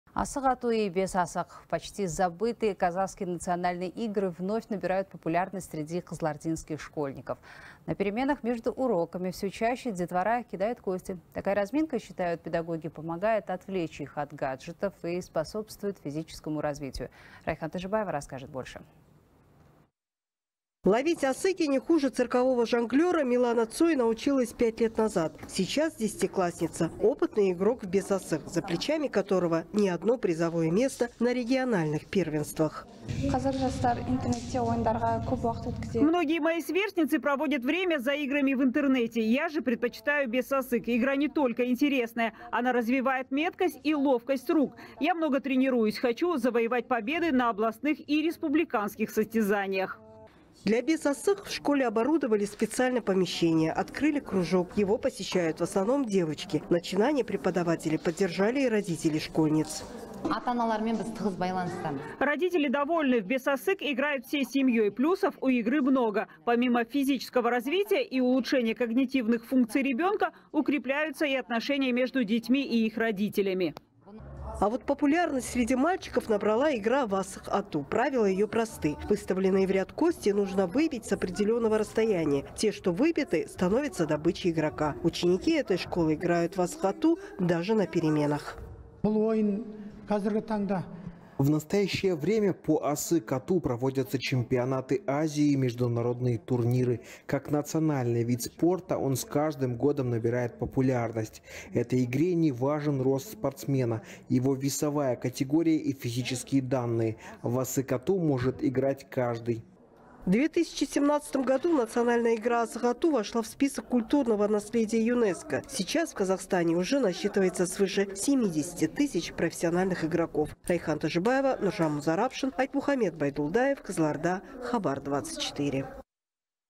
«Асык ату» и «Бес асык»: игра в кости набирает популярность среди кызылординских школьников
«Асык-ату» и «Бес асык» почти забытые казахские национальные игры вновь набирают популярность среди кызылординских школьников, передает корреспондент «Хабар 24». На переменах между уроками всё чаще детвора кидает кости. Такая разминка, считают учителя, помогает отвлечь их от гаджетов и способствует физическому развитию.